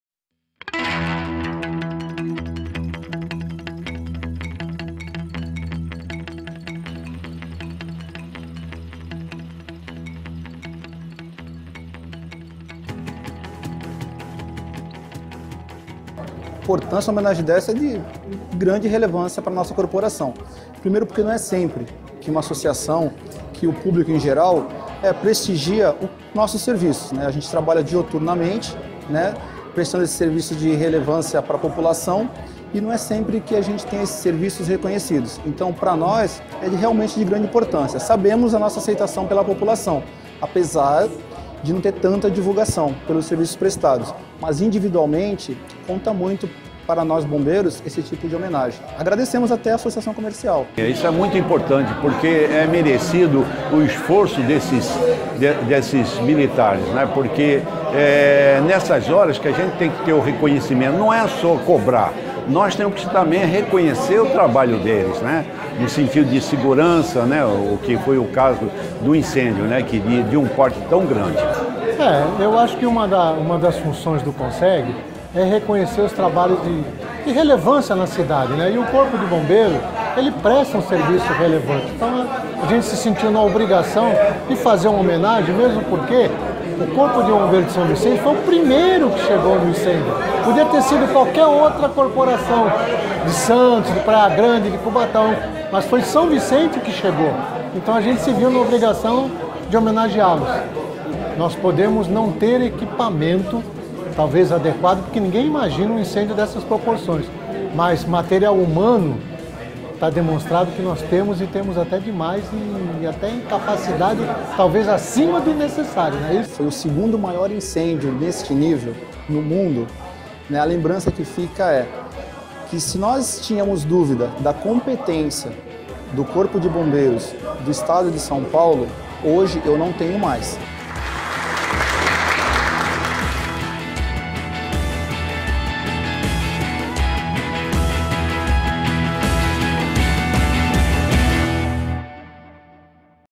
Associação Comercial de São Vicente homenageia bombeiros que atuaram no incêndio da Alemoa
Na última quinta-feira (11/06), o 1 º Conseg e a ACIESV prestaram homenagem aos bombeiros de São Vicente que atuaram no incêndio da Alemoa, em Santos. Confira o vídeo do evento!